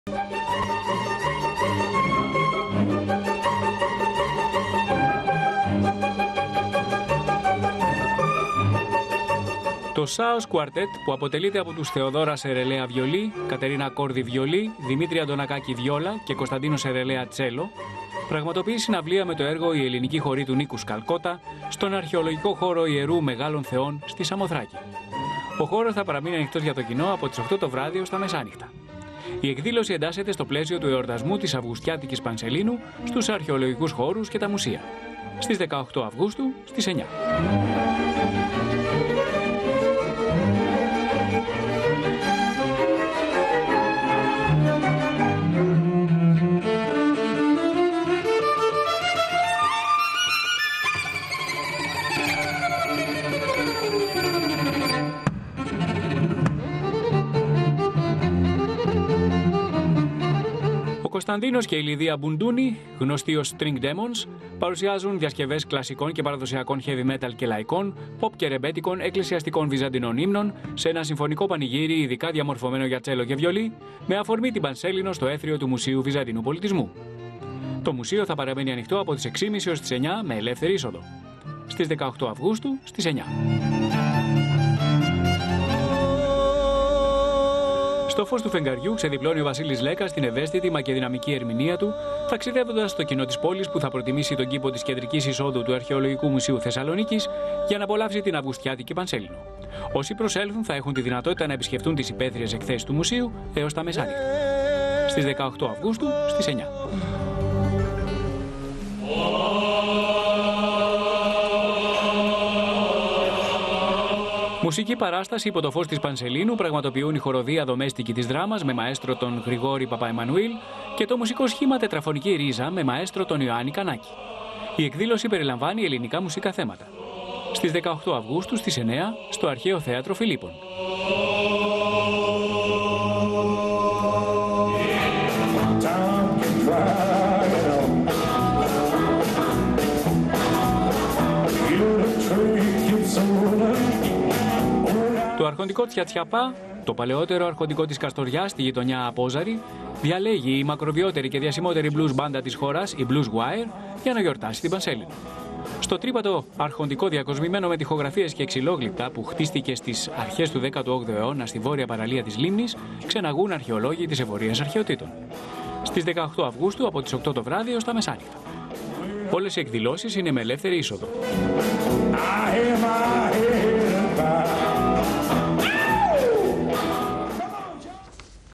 Πολιτιστική ατζέντα 16/08/2016
ΟΙ SAOS QUARTET ΣΤΟ ΙΕΡΟ ΜΕΓΑΛΩΝ ΘΕΩΝ ΣΑΜΟΘΡΑΚΗ

ΟΙ STRING DEMONS ΣΤΟ ΜΟΥΣΕΙΟ ΒΥΖΑΝΤΙΝΟΥ ΠΟΛΙΤΙΣΜΟΥ

Ο ΒΑΣΙΛΗΣ ΛΕΚΚΑΣ ΣΤΟ ΑΡΧΑΙΟΛΟΓΙΚΟ ΜΟΥΣΕΙΟ ΘΕΣΣΑΛΟΝΙΚΗΣ

ΟΙ ΔΟΜΕΣΤΙΚΟΙ ΤΗΣ ΔΡΑΜΑΣ ΣΤΟ ΑΡΧΑΙΟ ΘΕΑΤΡΟ ΦΙΛΙΠΠΩΝ

ΟΙ BLUES WIRE ΣΤΟ ΑΠΟΖΑΡΙ ΚΑΣΤΟΡΙΑ

ΕΠΙΜΕΛΕΙΑ ΣΤΑΥΡΟΣ ΠΑΝΟΥΣΗΣ